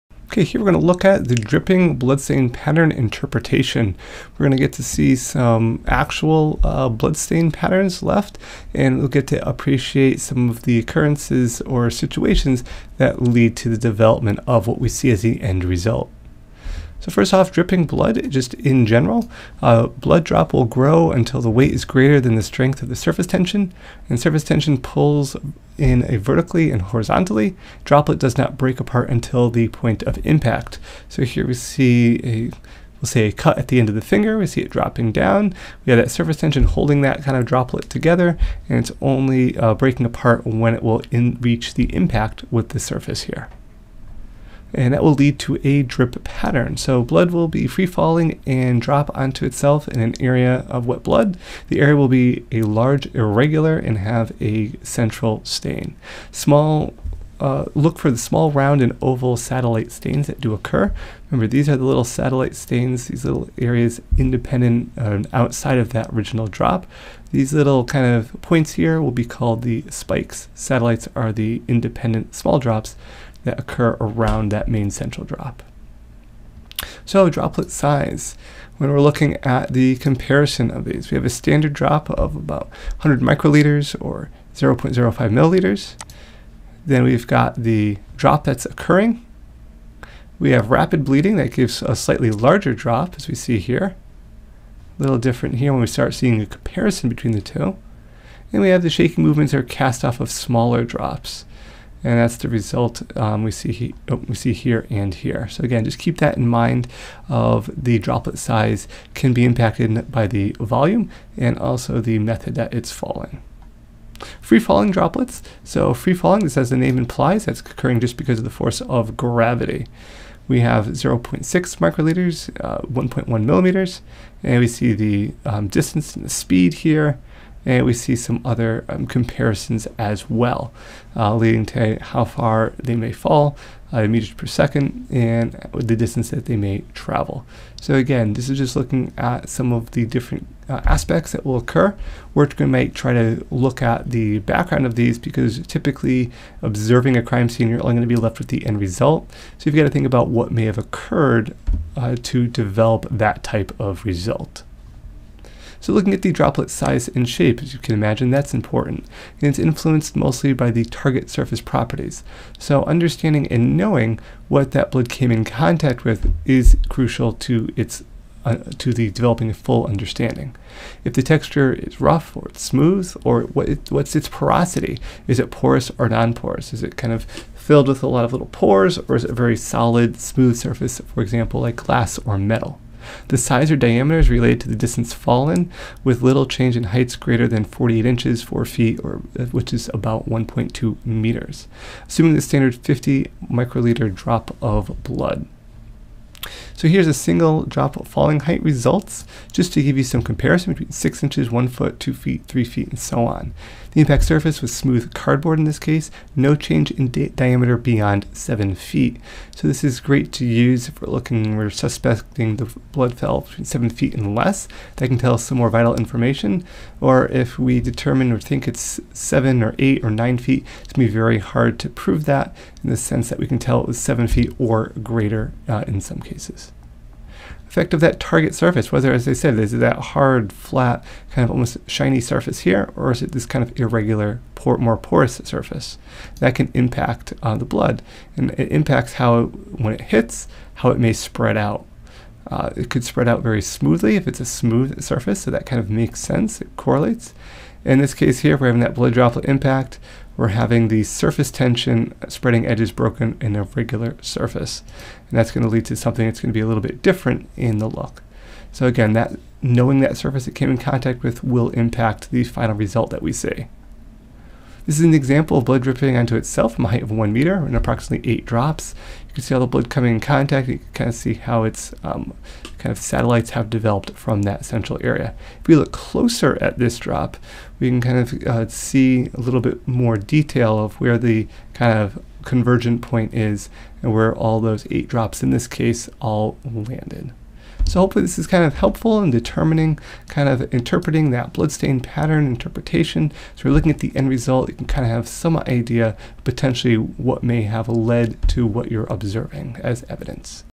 Dripping Bloodstain Pattern Interpretation
Dripping Blood
Blood drop will grow until the weight is greater than the strength of the surface tension
Surface tension pulls in vertically and horizontally
Droplet does not break-up until impact

Drip Pattern

Blood will be free-falling and drop onto itself in an area of wet blood
The area will be large irregular and have a central stain
Look for small round and oval satellite stains

Bloodspot Size and Shape
Influenced mostly by the target surface properties
Texture (rough or smooth) and porosity (porous or non porous)
Size (diameter) is related to distance fallen, with little change at heights greater than 48”/4ft./1.2m
Assuming standard 50 ul drop of blood
Effect of Target Surface

*Due to the description character limit the full work cited for "Dripping Bloodstain Pattern Interpretation" can be viewed